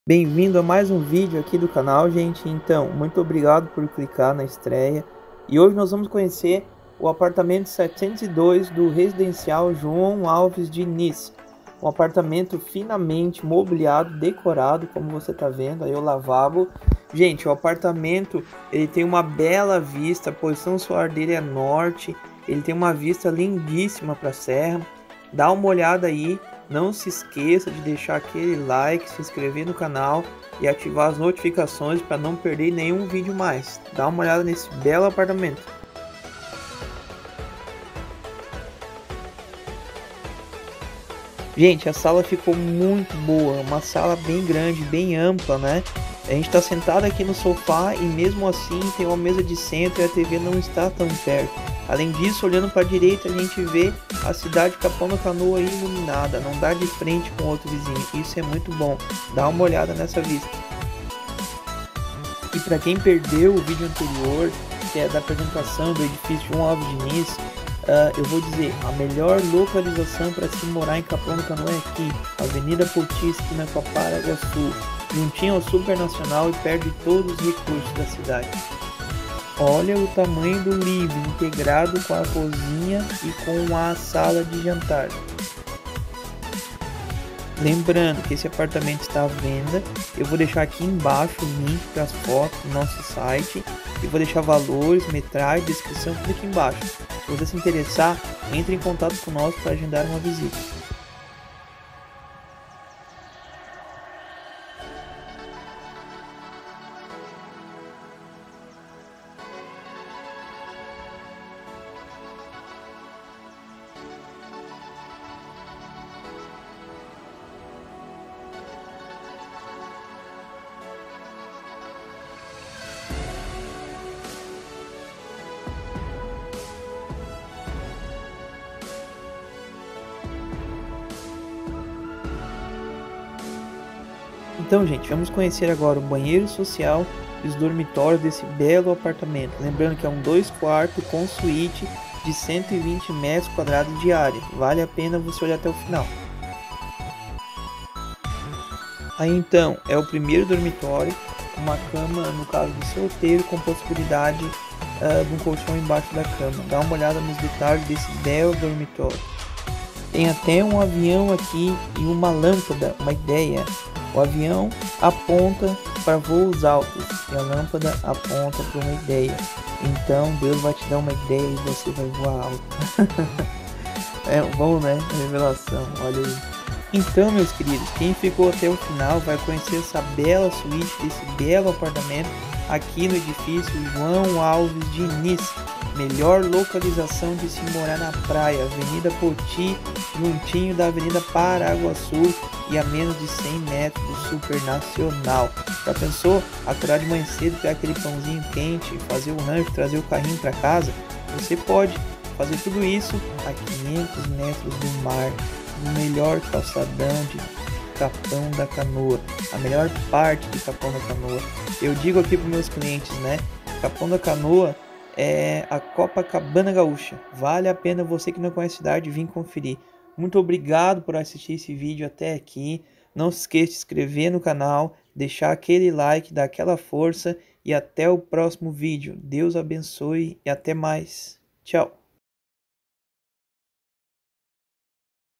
APE FINAMENTE DECORADO COM 120M² - LINDA VISTA
702 - ED JOÃO ALVES DINIZ  - R$ 950.000,00

Lindo apartamento finamente mobiliado e decorado, composto de 2 amplos dormitórios sendo uma suíte, ampla sala de estar e jantar, cozinha estilo americana, área de serviço, churrasqueira, com linda vista mar serra e lagoa, localização nobre na cidade de Capão da Canoa, junto ao super nacional, e de frente para avenida poti, condição especial de pagamento confira agora mesmo!

Ligue agora mesmo agende a sua visita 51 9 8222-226 whats ou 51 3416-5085